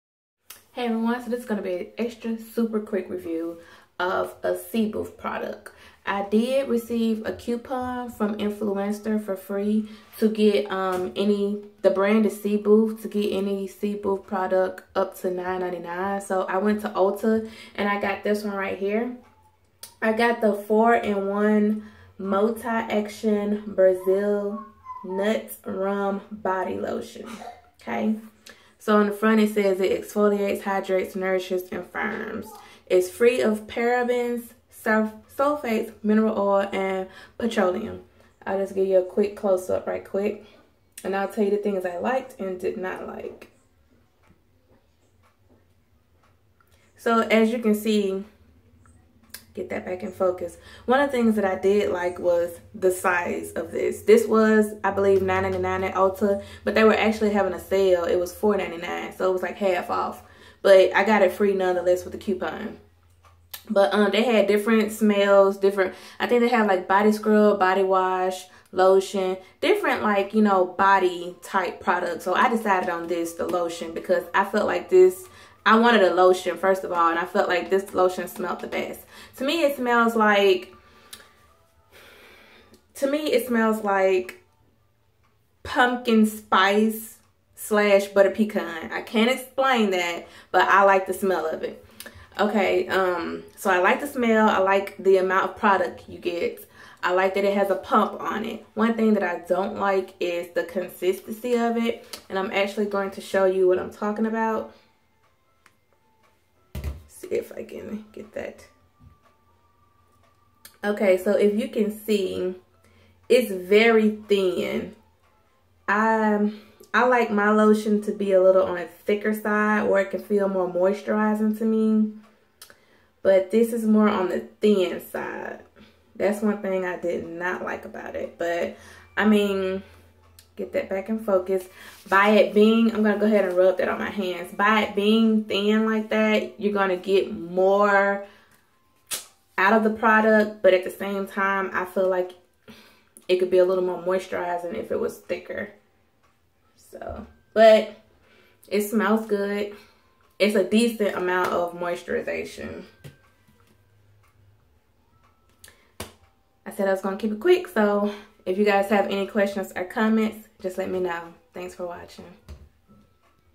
Review C.Booth Brazil Nut Rum Body Lotion
I did recieve this product free from influenster for my person review.